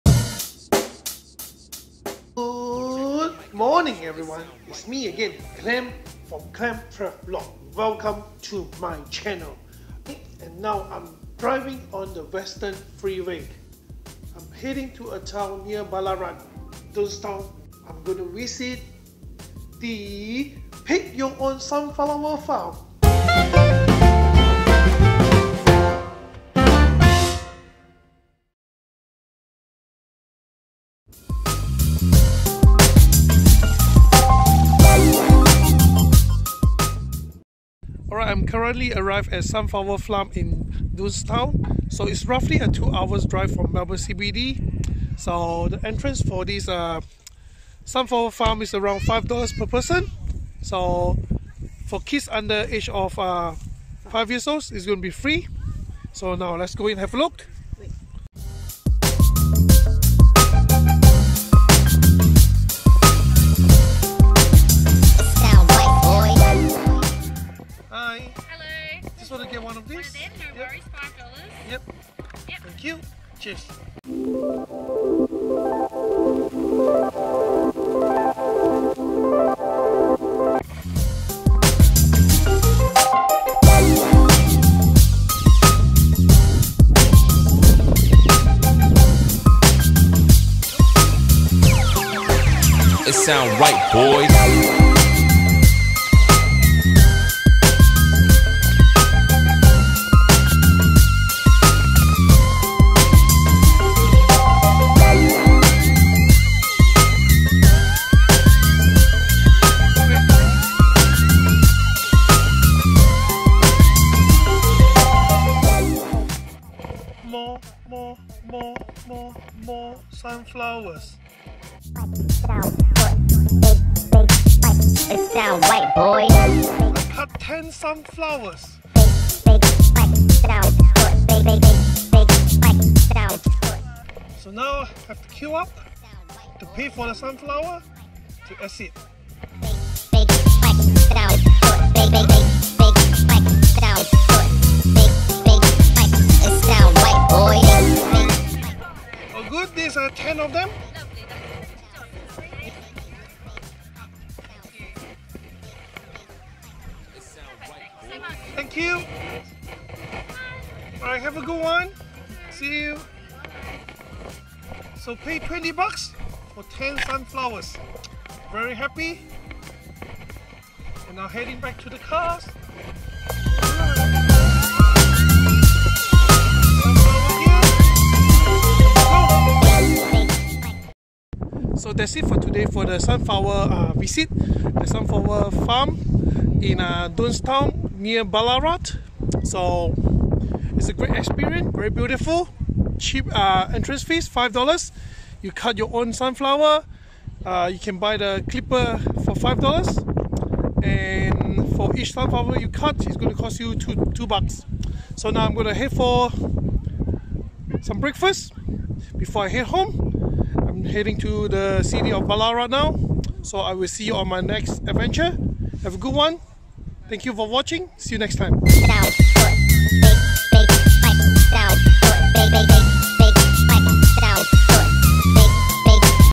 vlog #61: Visited "Pick Your Own Sunflowers" in Dunnstown
After weeks of recommendations from friends. I decided to visit one of the sunflower farms in Victoria. Located not far from the City of Ballarat, roughly 2 hours drive from Melbourne CBD via Western Freeway.

The entrance fees are $5 per adult and free for kids under five years old. For every sunflowers you picked will be charged $2 per flower.

Howards Road
Dunnstown VIC 3352
GPS coordinates 37°36'56.6"S 143°58'09.8"E